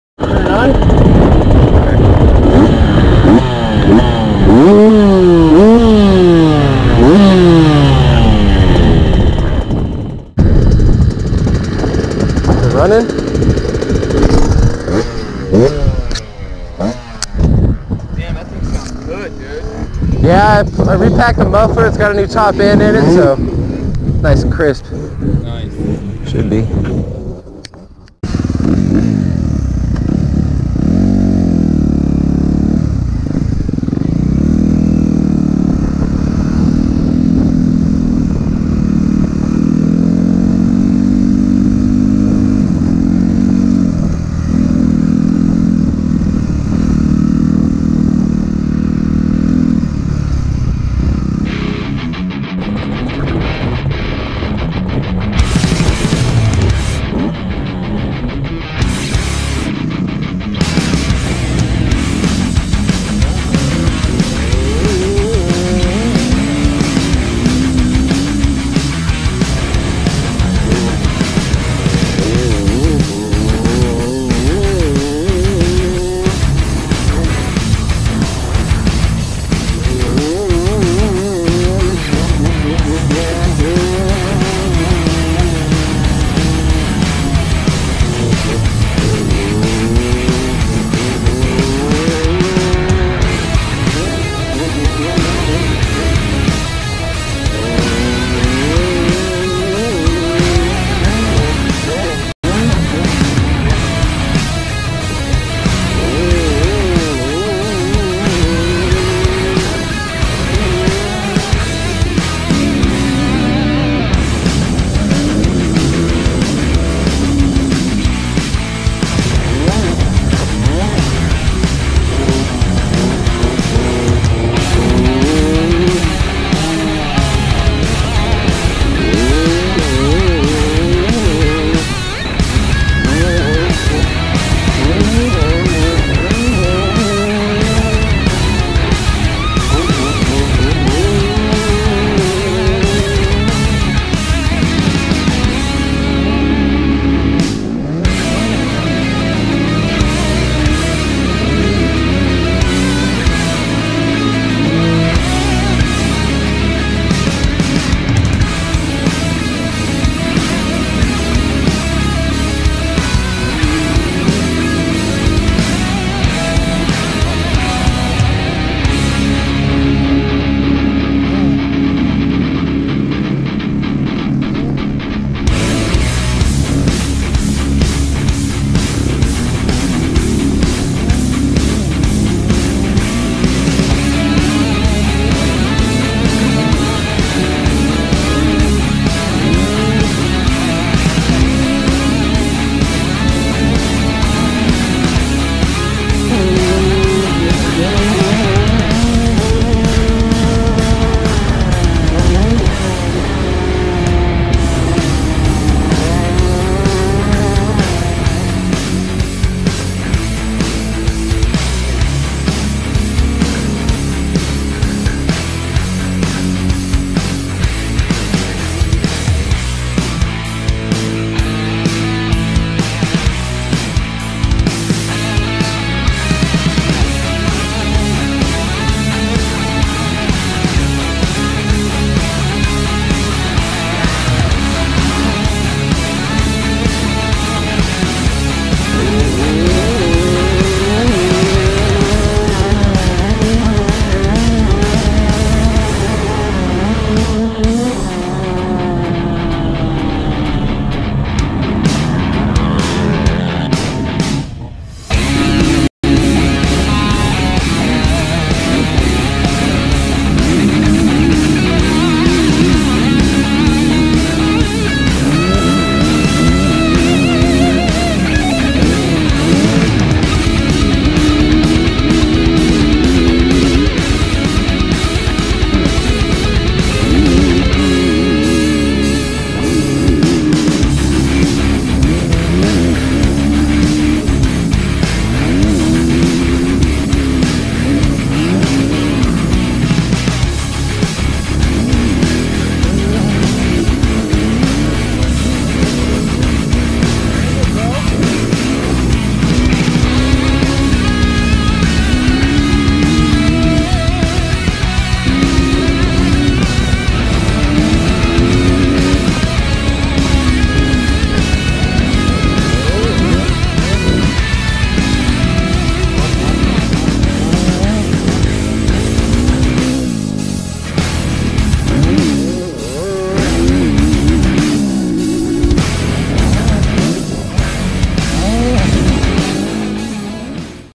Sending it at Aztec Raceway with Dad!
We went back to Aztec Raceway to ride and Dad jumped on his friends RMZ 250 again and tooks some laps with me! I finally started jumping the finish line jump and started sending it at Aztec Raceway with Dad!  Dont mind Dads GoPro chesty as it was a little loose and shaky at times, but still some good footage.